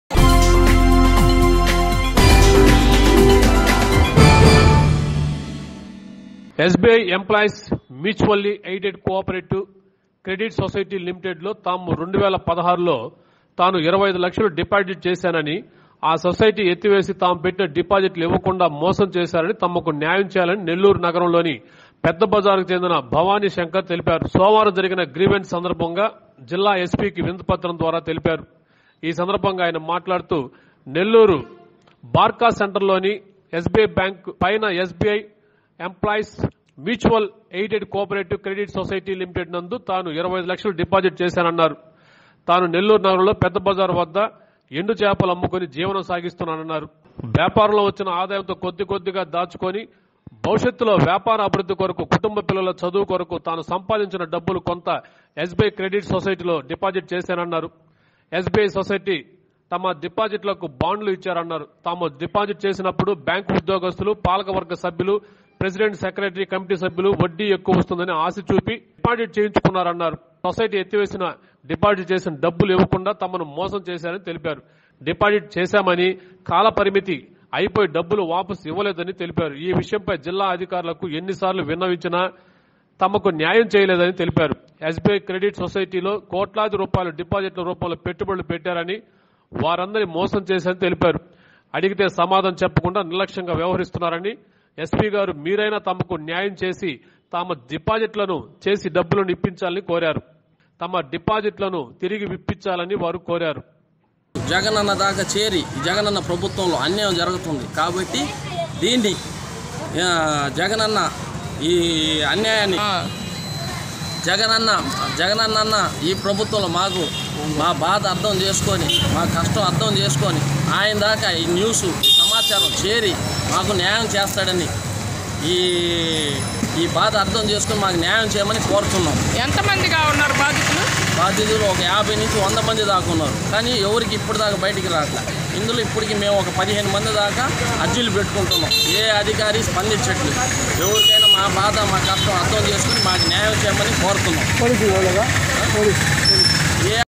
యస్ బి ఐ ఎంప్లాయి స్ మ్యాచ్ వల్లి ఎయిడెడ్ కోపరెటివ్ సొసైటీ మోసం నుండి న్యాయం చేయండి
Nellore News